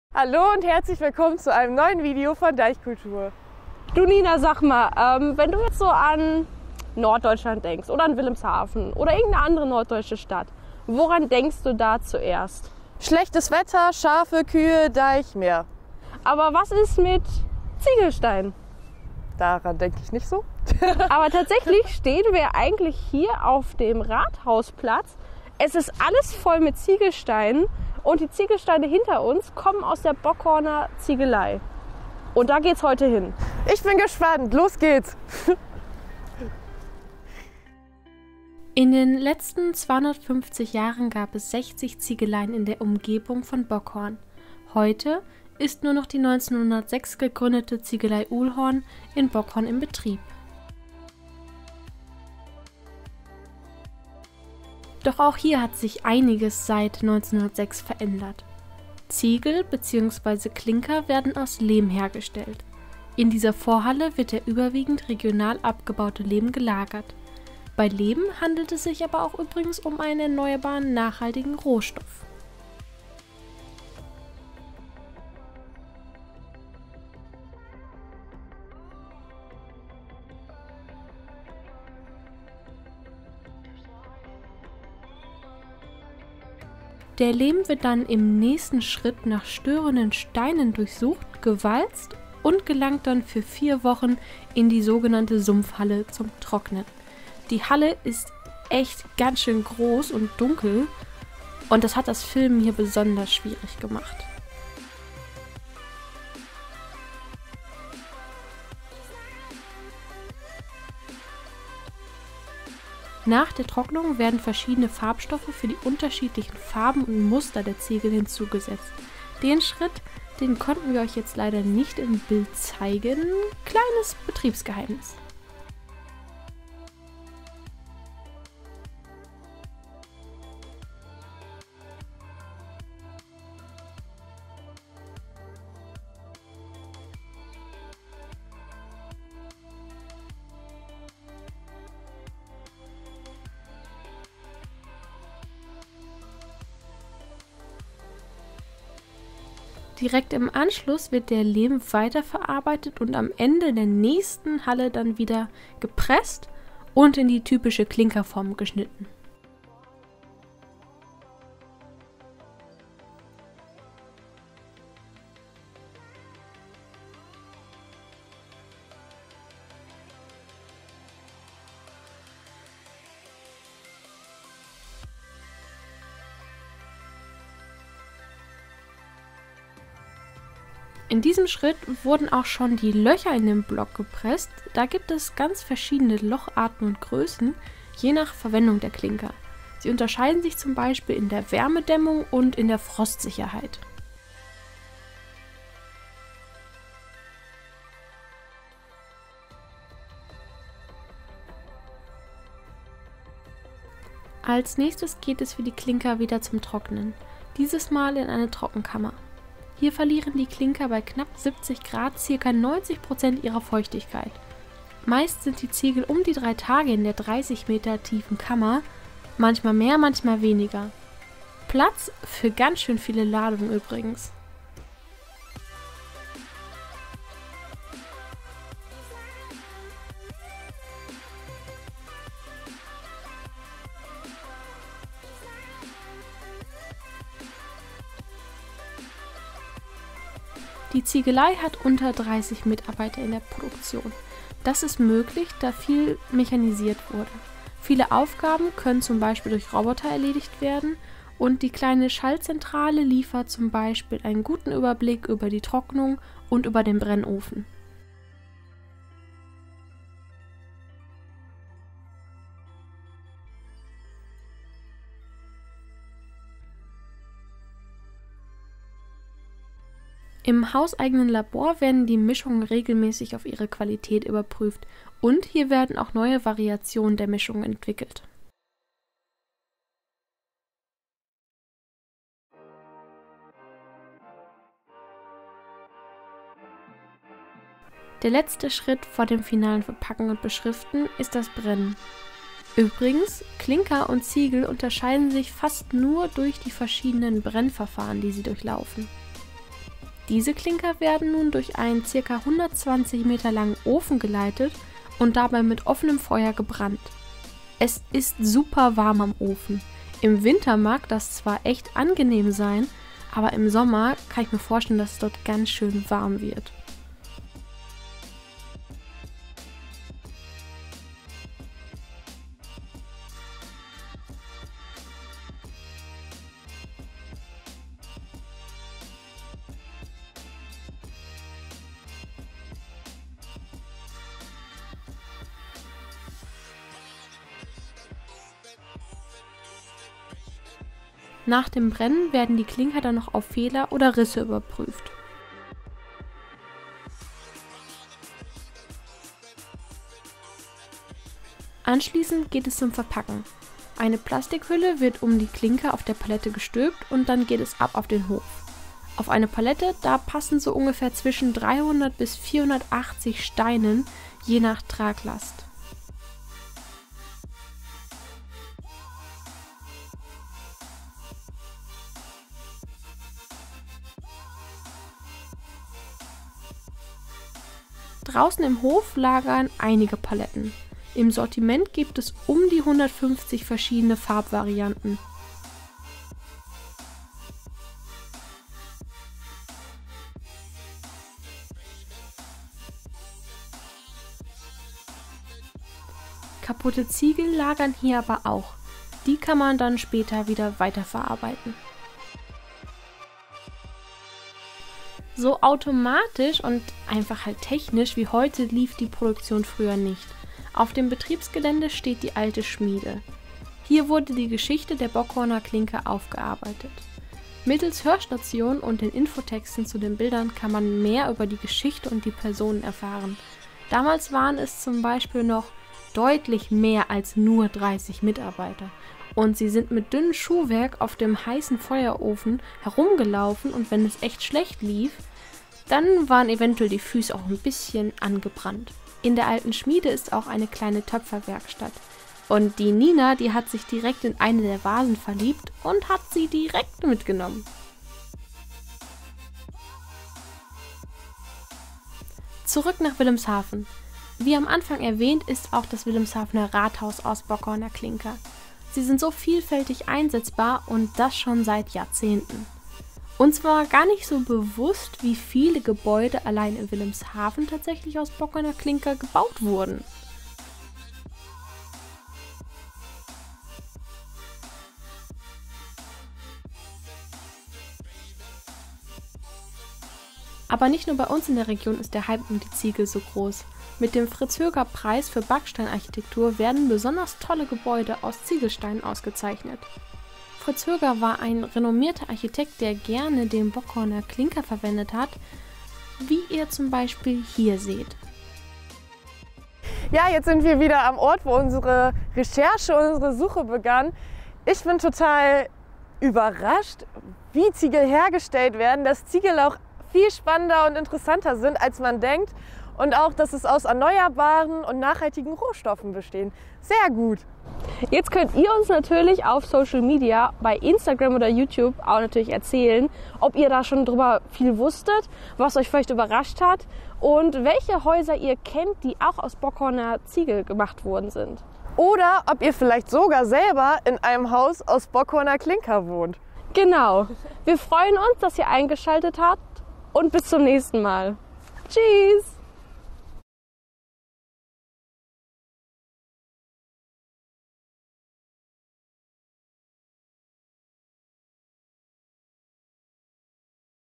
Zu Besuch in der Bockhorner Ziegelei
Habt ihr euch schon mal gefragt, wie Ziegelsteine gemacht werden? Und warum sagen manche Leute Klinker statt Ziegel? Ganz schön viele Fragen, die man sich da stellt.

Deswegen haben wir uns mal auf den Weg nach Bockhorn gemacht!
Hier haben wir eine Menge über die Bockhorner Klinker gelernt und euch das Wichtigste zusammengefasst.

Wenn ihr noch mehr Fragen habt, dann könnt ihr auch gerne auf der Seite der Ziegelei gehen und euch dort ein bisschen umschauen.